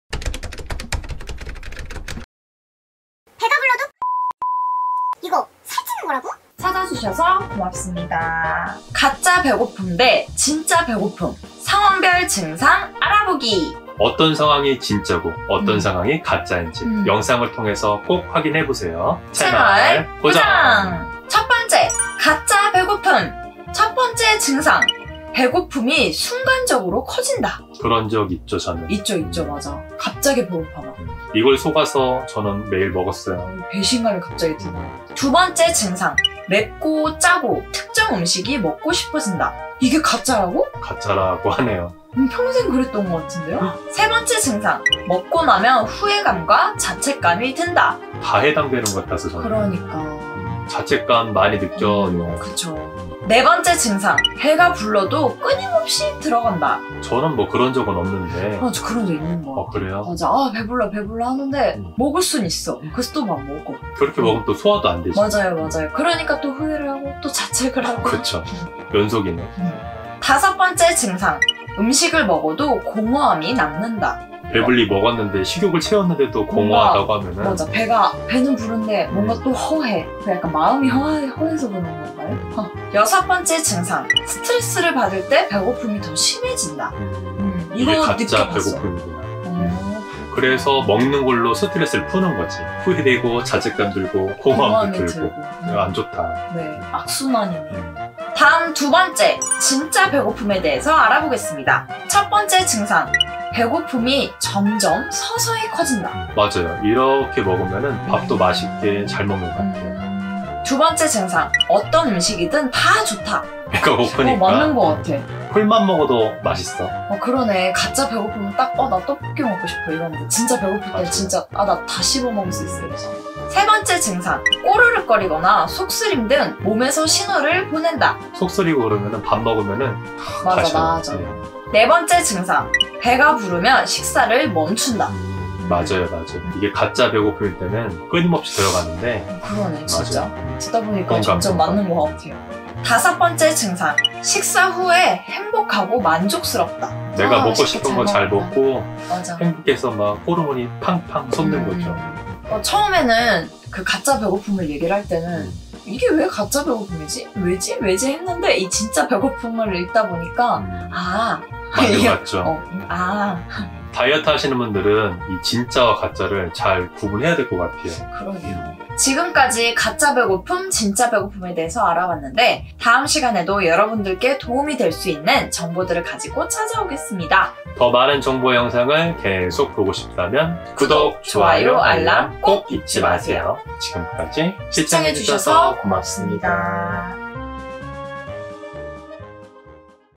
가짜배고픔 vs 진짜배고픔 상황별 증상 알아보기
내가 왜 살이 찌는지 이유를 알게 되었다..